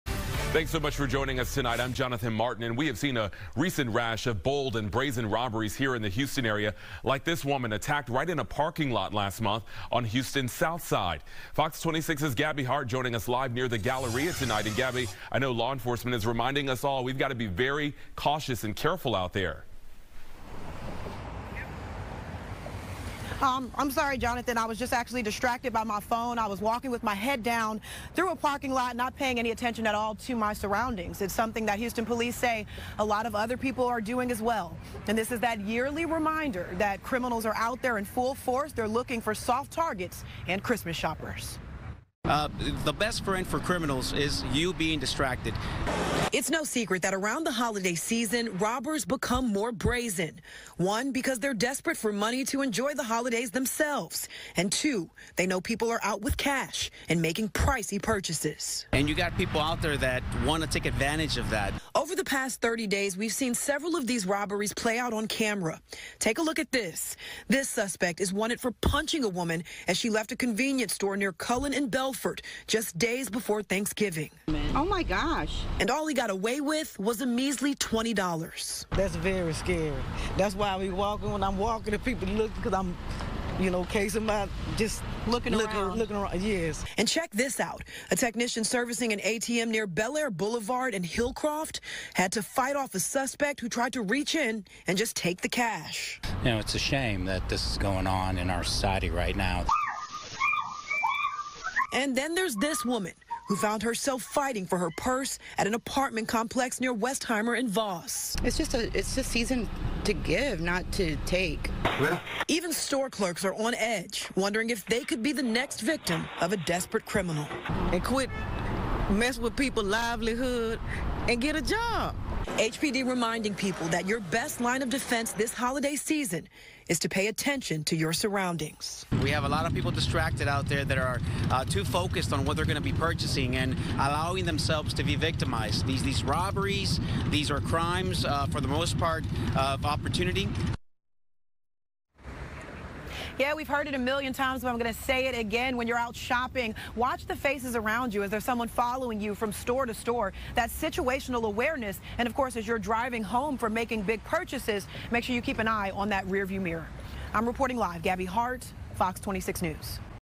HPD warning shoppers, residents to be aware of your surroundings
FOX 26 Reporter Gabby Hart spoke with shoppers and police about how they should remain aware of their surroundings during the holiday season.